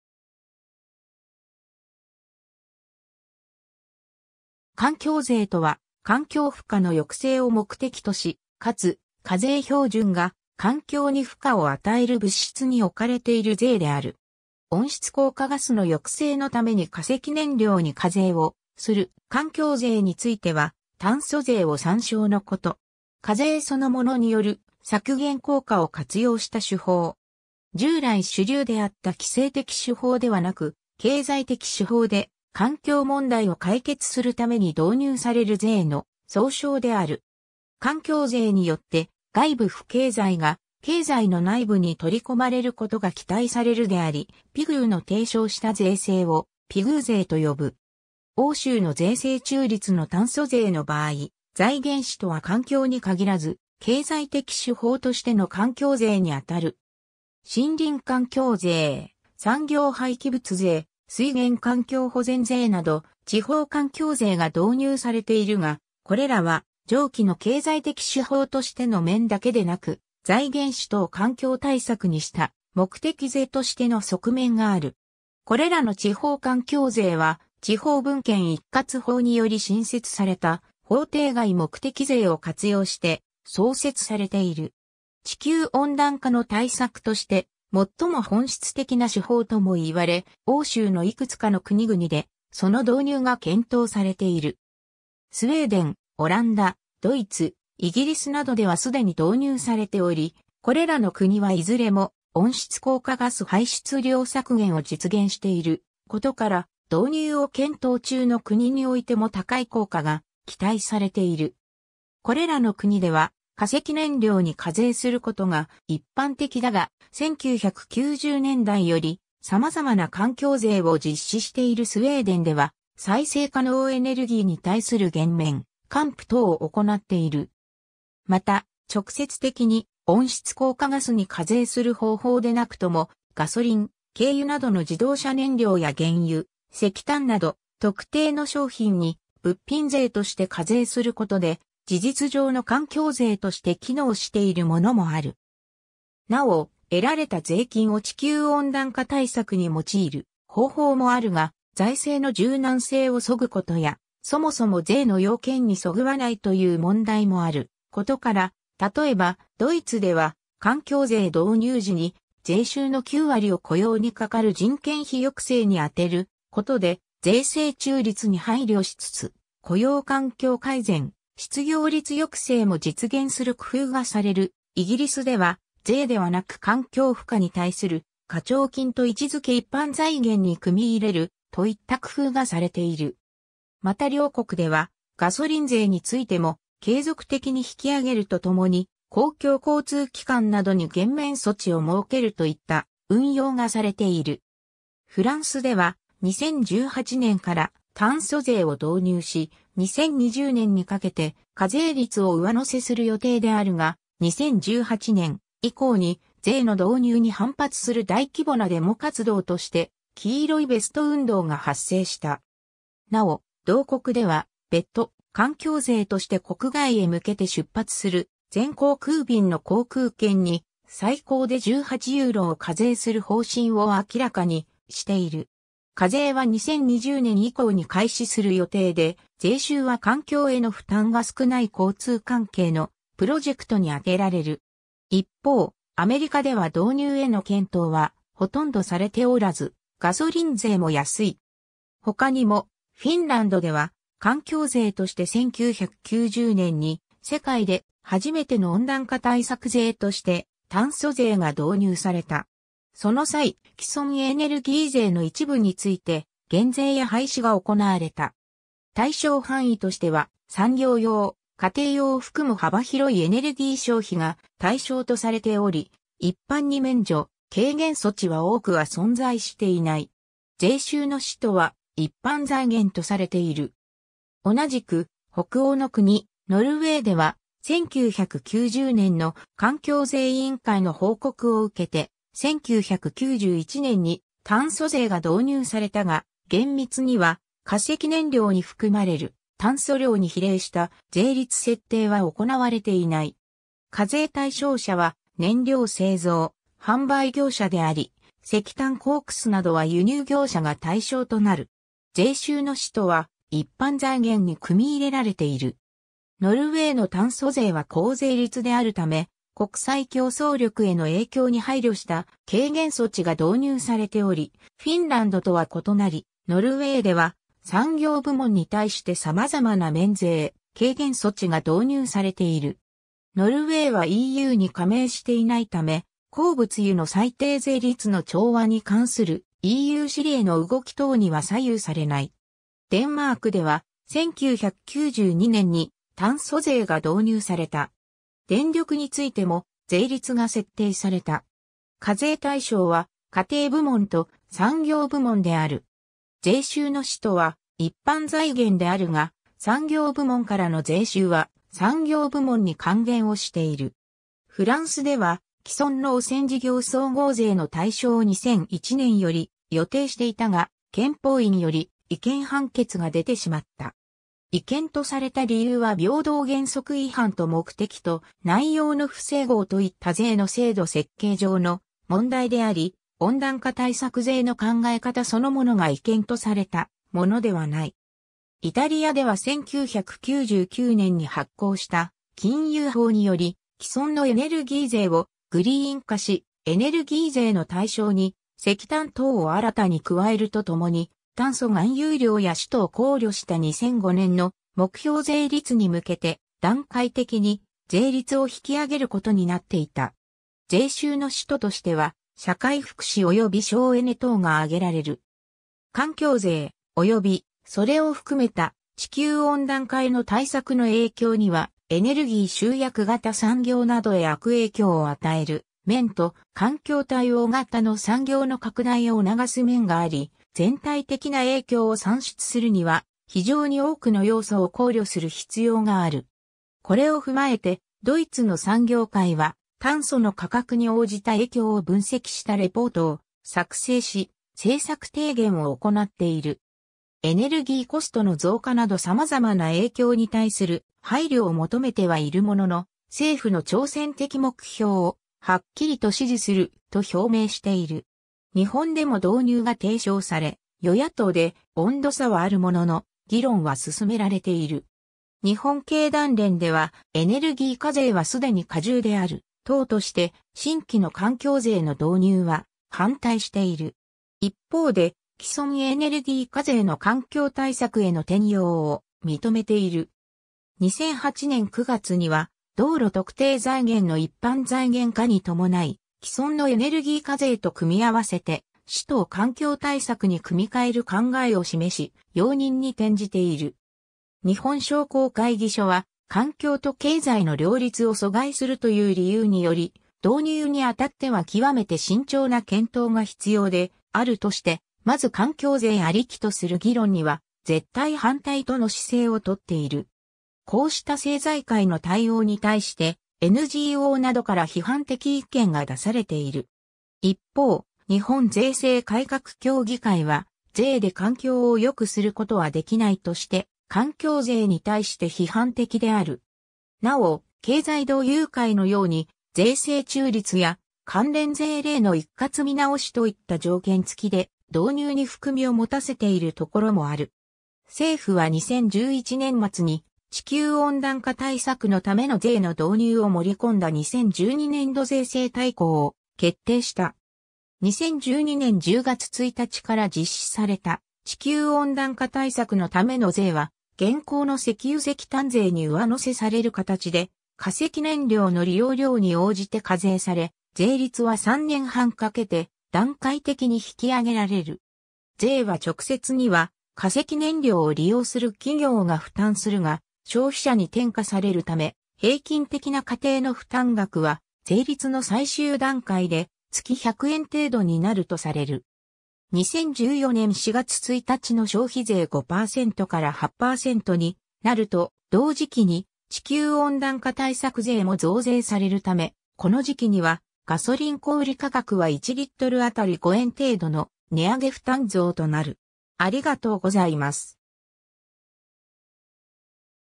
環境税
環境税（かんきょうぜい、environmental tax）とは、環境負荷の抑制を目的とし、かつ、課税標準が環境に負荷を与える物質に置かれている税である。
 温室効果ガスの抑制のために化石燃料に課税をする環境税については、炭素税を参照のこと。
 課税そのものによる削減効果を活用した手法。
従来主流であった規制的手法ではなく、経済的手法で環境問題を解決するために導入される税の総称である。
環境税によって外部不経済が経済の内部に取り込まれることが期待される（課税による外部不経済を市場内部へ取り込むことを主張したのは、アーサー・セシル・ピグー（1920年）であり、ピグーの提唱した税制をピグー税とよぶ）。
欧州の税制中立の炭素税の場合、財源使途は環境に限らず、経済的手法としての環境税にあたる。
 森林環境税・産業廃棄物税・水源環境保全税など、地方環境税が導入されているが、これらは上記の経済的手法としての面だけでなく、財源使途を環境対策にした目的税としての側面がある。
これらの地方環境税は、地方分権一括法により新設された法定外目的税を活用して、創設されている。
 地球温暖化の対策として最も本質的な手法とも言われ、欧州のいくつかの国々でその導入が検討されている。
スウェーデン、オランダ、ドイツ、イギリスなどでは既に導入されており、これらの国はいずれも温室効果ガス排出量削減を実現している（京都議定書#各国の取組状況を参照）ことから、導入を検討中の国においても高い効果が期待されている。
これらの国では化石燃料に課税することが一般的だが、1990年代より様々な環境税を実施しているスウェーデンでは再生可能エネルギーに対する減免・還付等を行っている。
 また、直接的に温室効果ガスに課税する方法でなくとも、ガソリン・軽油などの自動車燃料や原油、石炭など特定の商品（化石燃料）に物品税（個別消費税）として課税することで、事実上の環境税として機能しているものもある。
 なお、得られた税金を地球温暖化対策に用いる（特定財源とする）方法もあるが（日本の環境省はこの方式による炭素税導入を提案している）、財政の柔軟性を削ぐことや、そもそも税の要件（公平・中立や財源安定など）にそぐわないという問題もあることから、たとえばドイツでは環境税（炭素税）導入時に税収の 9割を雇用にかかる人件費抑制に充てる（具体的には社会保険料の縮減。残り 1割は環境対策に充てられている）ことで税制中立に配慮しつつ雇用環境改善・失業率抑制も実現する工夫がされる、イギリスでは税（Tax）ではなく環境負荷に対する課徴金（Levy）と位置付け（en:Climate Change Levy）一般財源に組み入れる、といった工夫がされている。
また両国ではガソリン税についても継続的に引き上げるとともに、公共交通機関などに減免措置を設けるといった運用がされている。
 フランスでは、2018年から炭素税を導入し、2020年にかけて課税率を上乗せする予定であるが、2018年以降に税の導入に反発する大規模なデモ活動として黄色いベスト運動が発生した。
なお、同国では、別途、環境税として国外へ向けて出発する全航空便の航空券に、最高で18ユーロ（ビジネスクラスの課税額）を課税する方針を明らかにしている。
課税は2020年以降に開始する予定で、税収は環境への負担が少ない交通関係のプロジェクトに充てられる。
 一方、アメリカでは導入への検討はほとんどされておらず、ガソリン税も安い。
 他にも、フィンランドでは環境税として1990年に世界で初めての温暖化対策税として炭素税が導入された。
その際、既存エネルギー税の一部について減税や廃止が行われた。
対象範囲としては産業用・家庭用を含む幅広いエネルギー消費が対象とされており、一般に免除・軽減措置は多くは存在していない。
税収の使途は一般財源とされている。
 同じく北欧の国、ノルウェーでは、1990年の環境税委員会の報告を受けて、1991年に炭素税が導入されたが、厳密には化石燃料に含まれる炭素量に比例した税率設定は行われていない。
課税対象者は燃料製造、販売業者であり、石炭コークスなどは輸入業者が対象となる。
税収の使途は一般財源に組み入れられている。
 ノルウェーの炭素税は高税率であるため、国際競争力への影響に配慮した軽減措置が導入されており、フィンランドとは異なり、ノルウェーでは産業部門に対して様々な免税・軽減措置が導入されている。
ノルウェーはEUに...